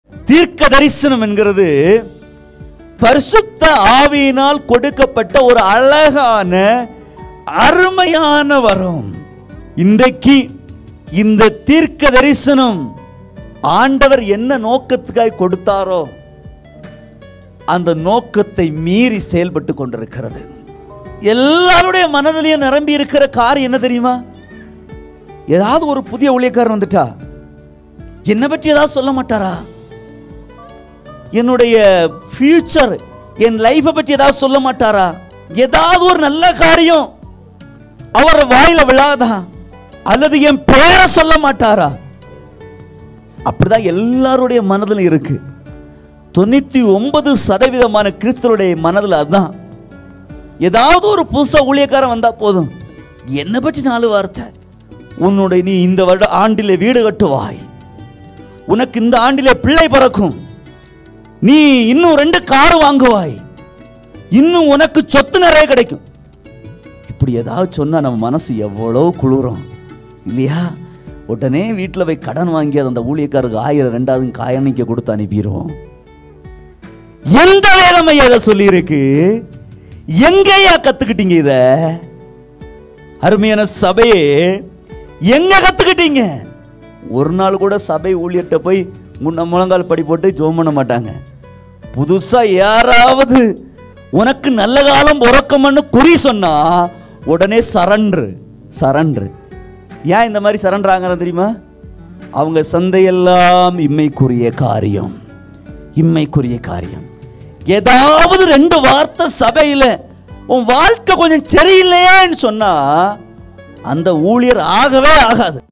பைபிள்: தீர்க்கதரிசனத்தின் ஆவி "இயேசு கிறிஸ்துவைப் பற்றின சாட்சி"| மற்ற யாவும் நிதானிக்க வேண்டியவை!
STATEMENT OF TRUTH:
“And I fell at his feet to worship him. And he said unto me, See thou do it not: I am thy fellowservant, and of thy brethren that have the testimony of Jesus: worship God:
-for the testimony of Jesus is the spirit of prophecy.”